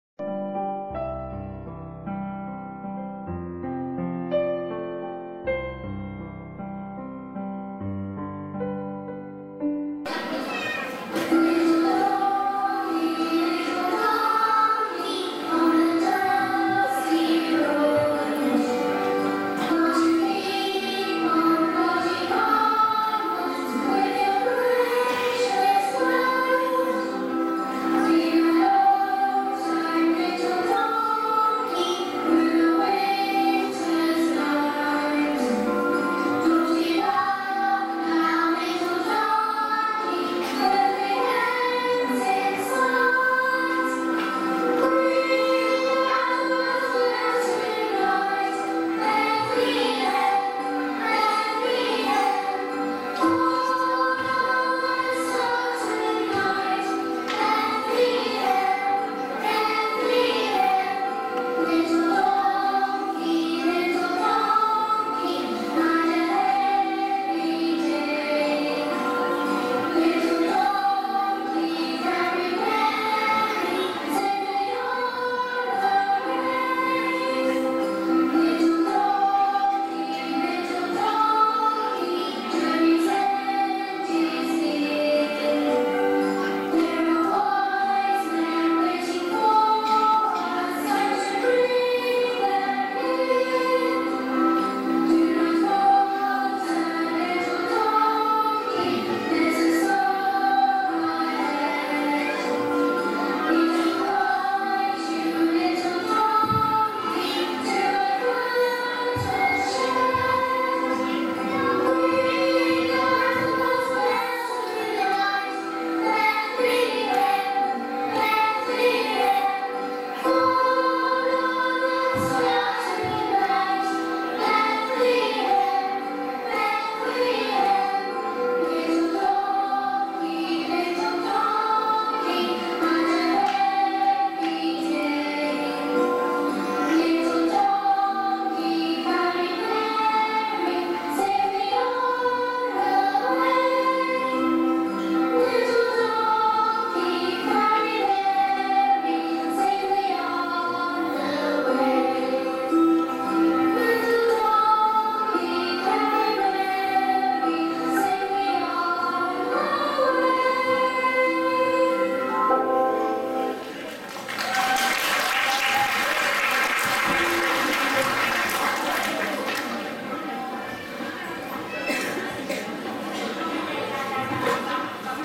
Christmas 2015 - Carols by Tiny Tots of St. Thomas IOC
Christmas 2015 - Carols by Tiny Tots of St. Thomas Indian Orthodox Church, Hemel Hempstead.